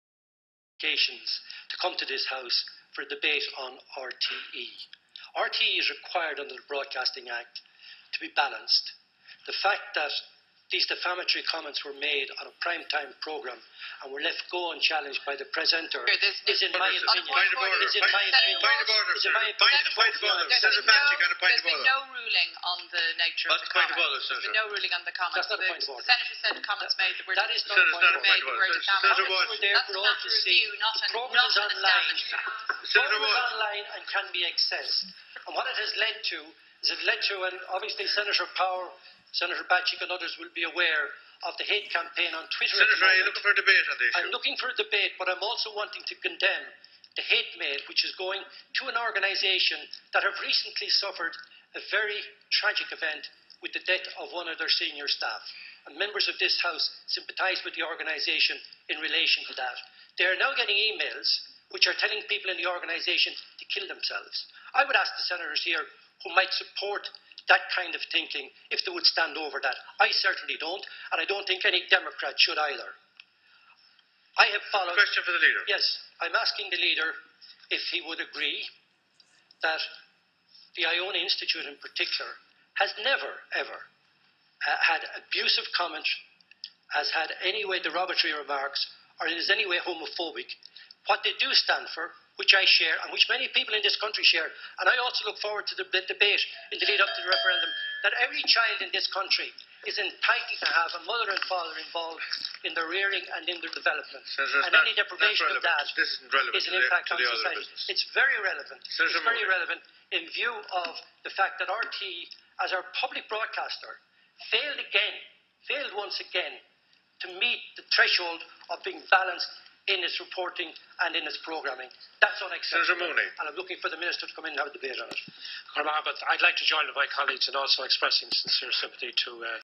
Senator Jim Walsh takes the Iona Institute's side.
Senator Jim Walsh defends the Iona Institute